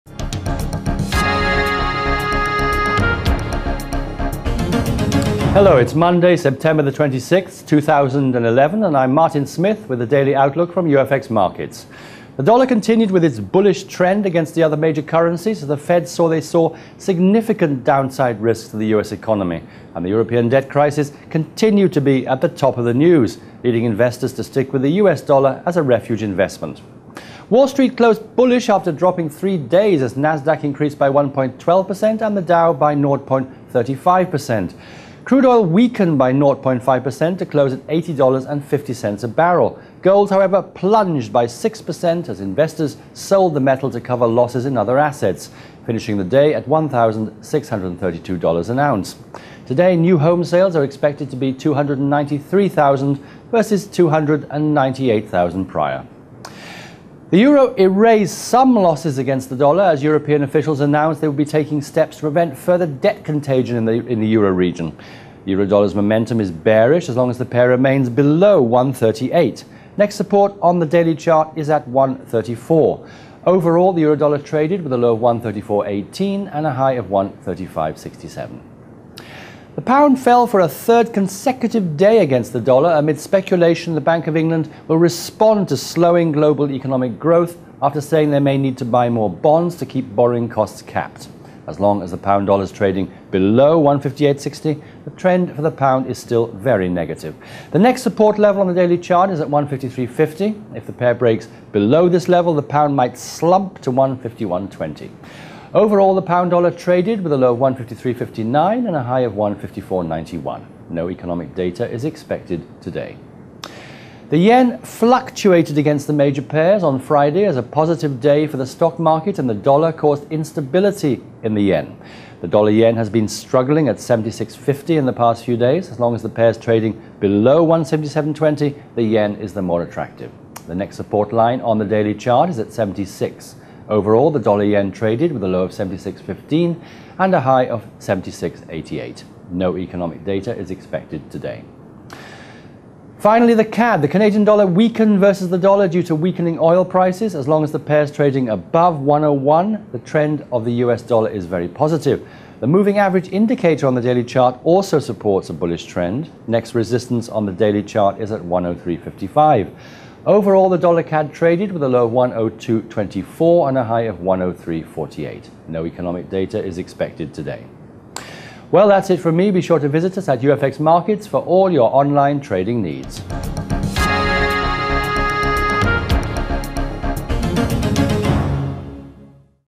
UFXMarkets -Daily Gold & Forex Trading News-26-September-2011
Gold (XAU) plunged by 6.0% as investors sold the precious metal to cover losses on other assets and finished at $1632 an ounce.